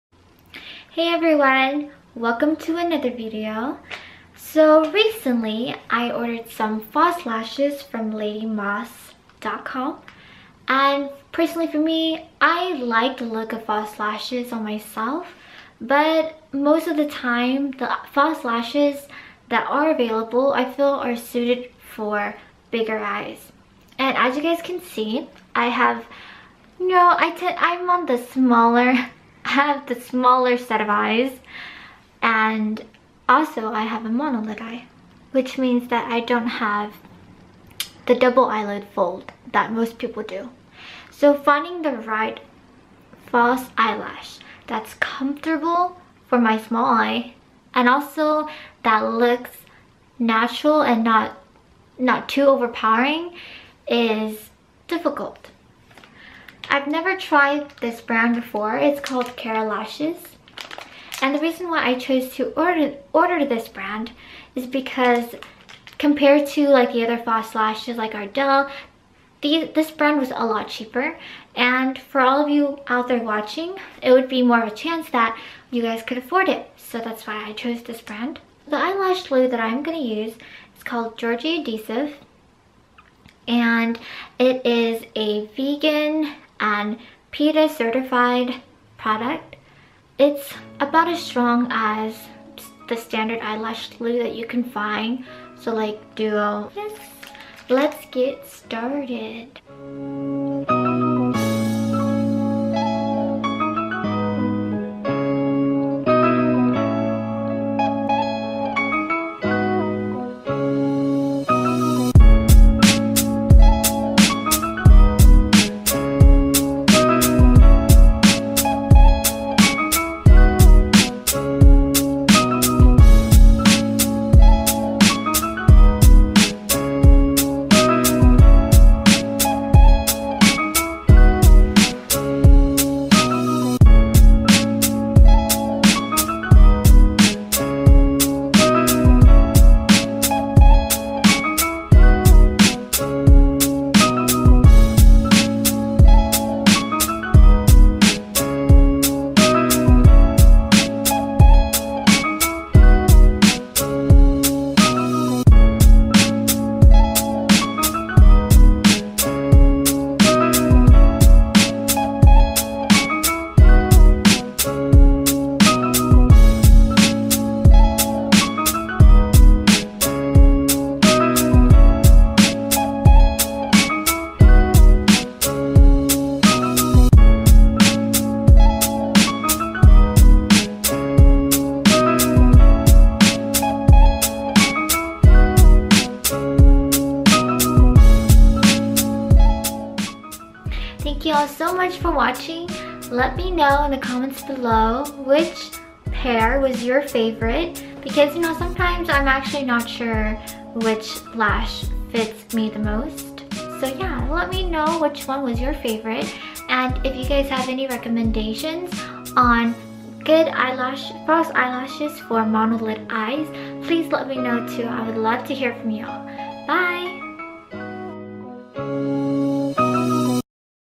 KARA Lashes - Monolid Eyes
Eyelashes can give your makeup look the perfect pop. But with small monolid eyes, it's often difficult to find the right pair of lashes. In this video, I review 5 pairs of KARA lashes. Overall, I am satisfied with them. I was expecting them to be bigger than how they're pictured on the website (i'm glad they weren't). The bands are thin and the lashes are lightweight. Those going for a more dramatic look may want to stack two lashes together since the lashes are thin. If you like these kinds of videos or have any suggestions, let me know! :)

Products Mentioned:

KARA Lashes #46, #213, #1, S1, #601
Georgie Black Faux Lash Adhesive

Monolid Eyes Makeup